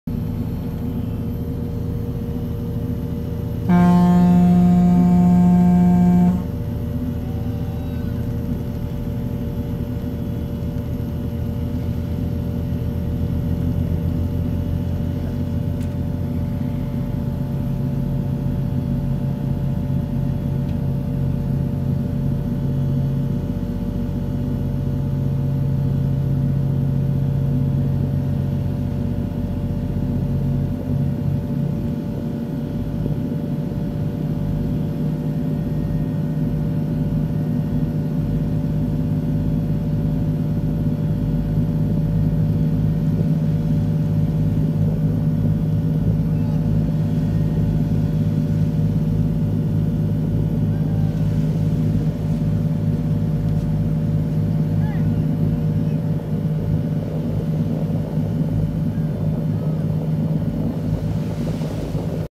Lintas Padang Bay - Lembar, Di hadang Jaring Nelayan jam 23.10 WITA,
Lintas padang bay - Lembar , sering terjadi susahnya saat melintasi trayek, dikarenakan jaring nelayan bertebaran saat malam
Hari, tidak terlihat oleh Perwira kapal, bagi nelayan sepantasnya mengisi lampu jaringnya biar terditeksi, kapal tidak bisa mengerem mendadak kalau sudah begin.   Saat meletakan jaring pukul 23.10 wita keadaan gelap gulita